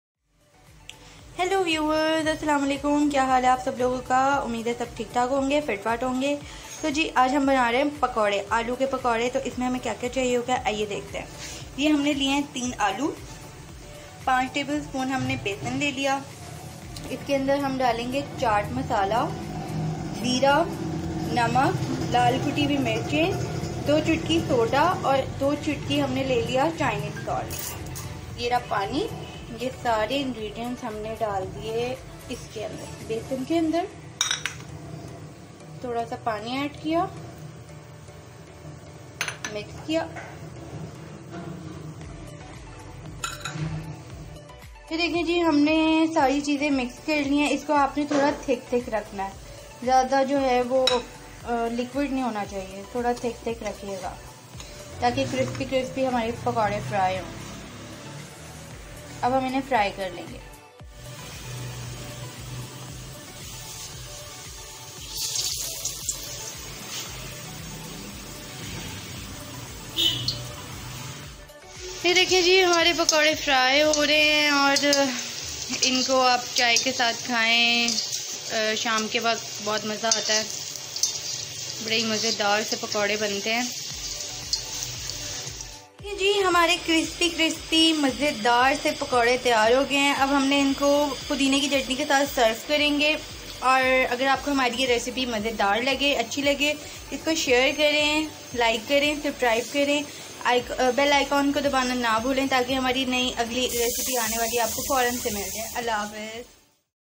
alooo k pakory
HOW  to make alo pakora
hello guys!
3 potatoes
5tbl spoon Basan
half tea apoon salt
half tea spoon chilli flacks
half tea spoon zeera
half tea spoon chat masala
2 chutki chiness salt
2chutki meatha soda